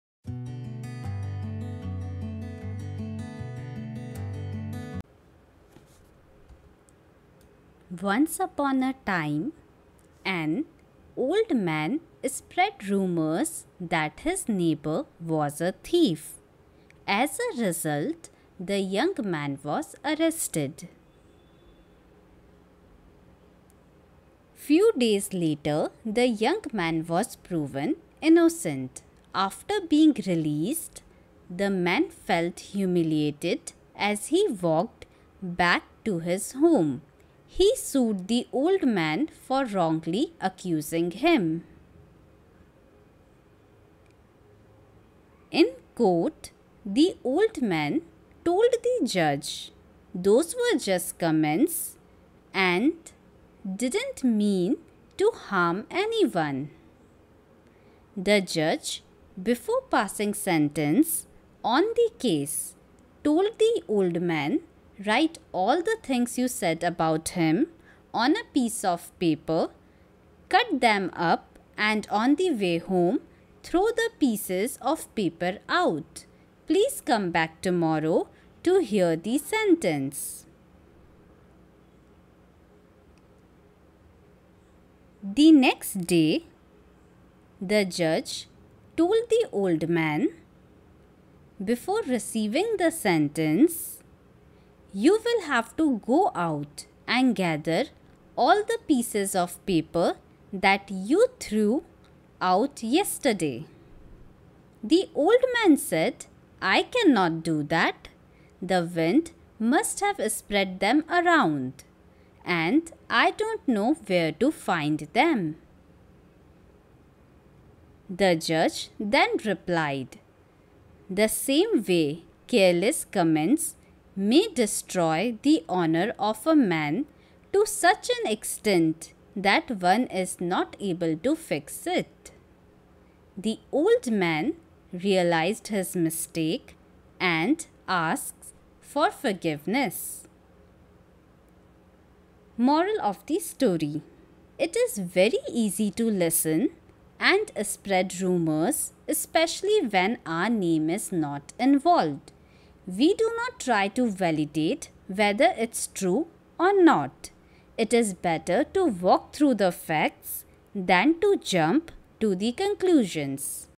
Short Inspirational Story | The Biggest Liar in the world is "They Say" | Never Spread Rumors
Enjoy the animated moral stories for kids and students. Build insights through a collection of short inspirational stories. These are fictional stories showcasing an important life lesson for people from all walks of life.